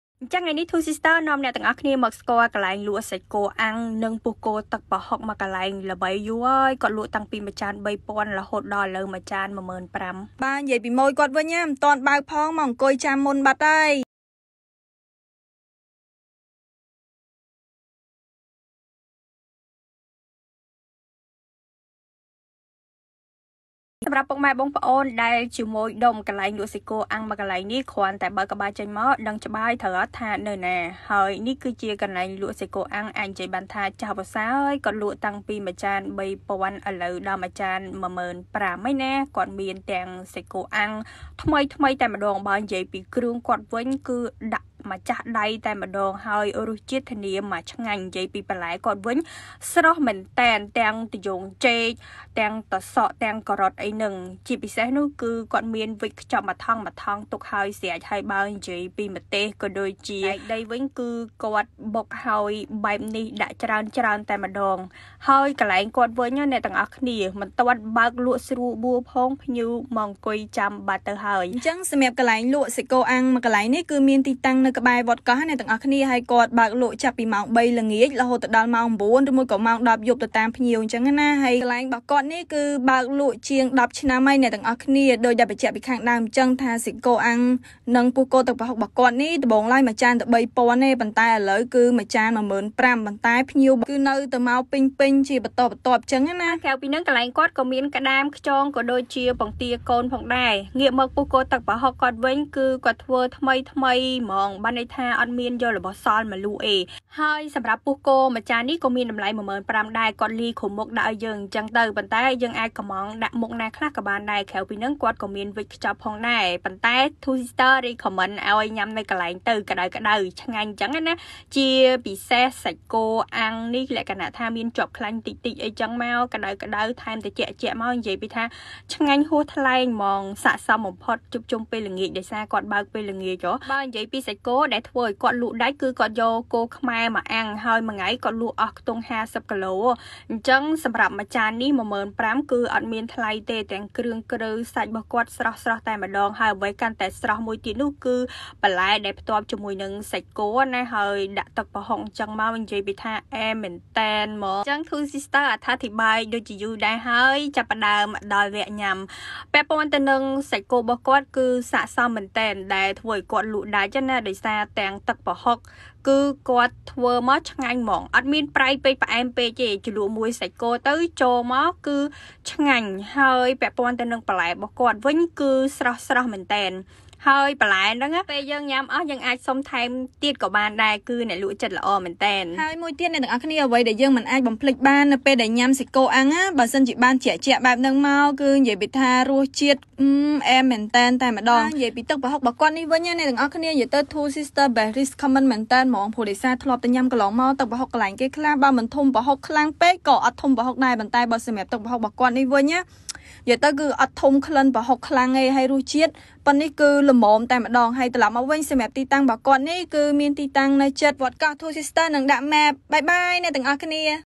សាច់គោអាំងជាប់ខ្លាញ់ ពោះគោទឹកប្រហុកម្តុំវត្តកោះ - Grilled Beef with Prahok Sauce | 2sister
ថ្ងៃនេះនរណាខ្លះទំនេរ មកបូកគីឡូជាមួយ Two Sisters! សាច់គោអាំងជាប់ខ្លាញ់ ពោះគោទឹកប្រហុកម្តុំវត្តកោះ

⏰បេីកលក់ពីម៉ោង 3:30 ល្ងាច ដល់ម៉ោង10 យប់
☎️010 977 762
📍ជាប់សាលាមតេ្តយ្យវត្តកោះ